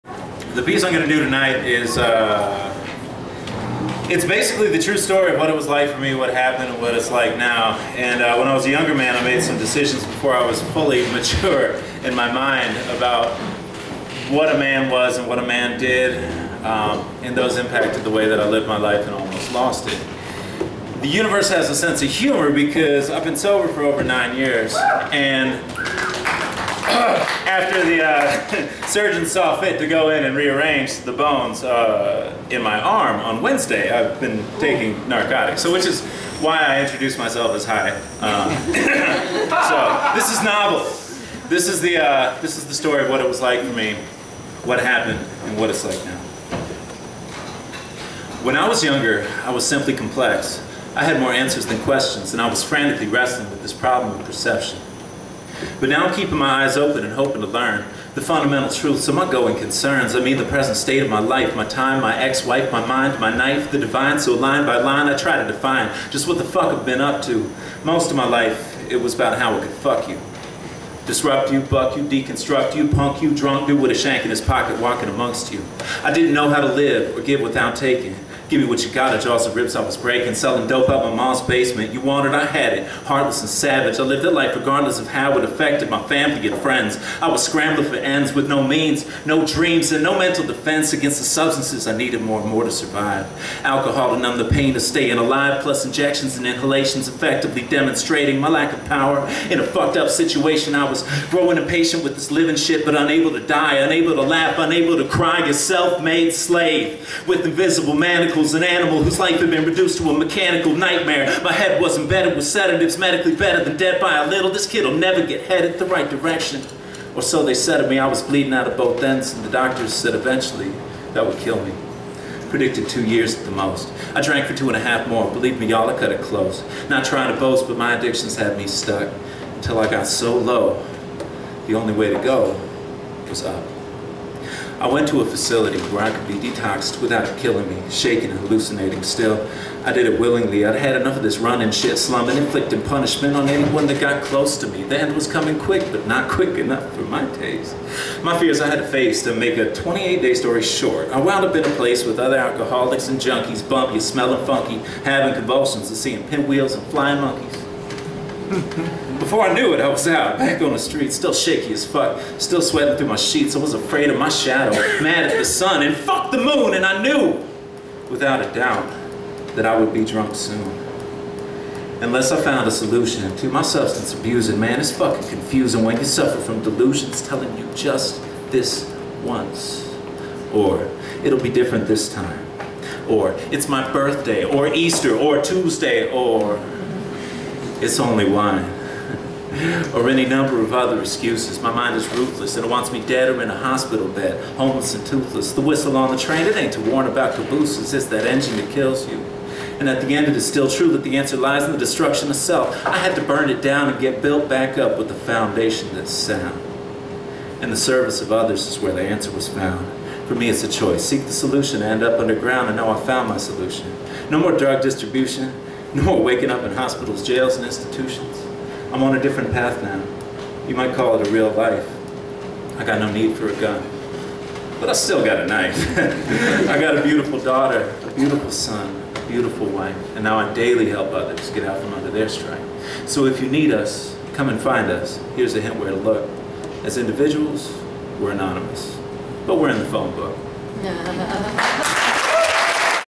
Andrew presents The Story
Andrew presents The Story at the Fort Collins First Friday Poetry Slam. 4/16